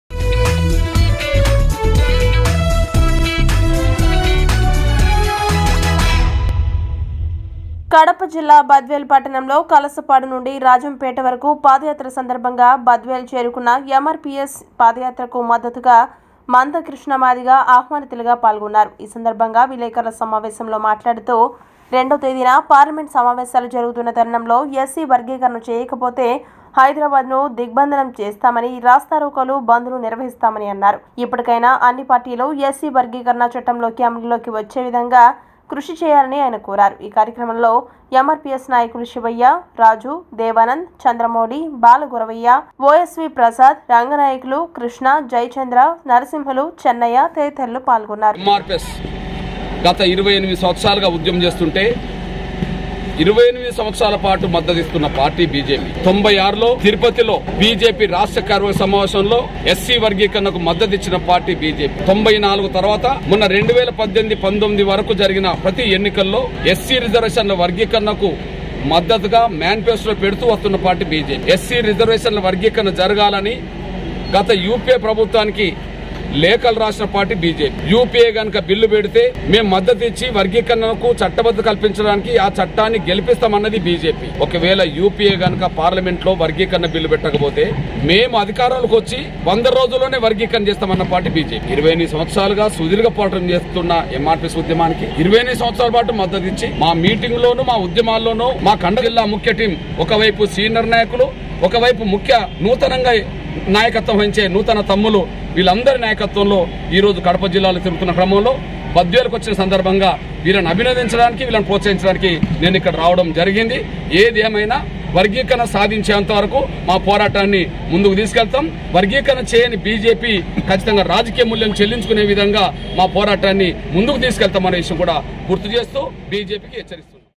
బద్వేల్|| SC వర్గీకరణ చేస్తానని మాట తప్పిన BJP ప్రభుత్వానికి తగిన గుణపాఠం తప్పదు: MRPS మంద కృష్ణ..
@truthnewstelugu బద్వేల్|| SC వర్గీకరణ చేస్తానని మాట తప్పిన BJP ప్రభుత్వానికి తగిన గుణపాఠం తప్పదు: MRPS మంద కృష్ణ..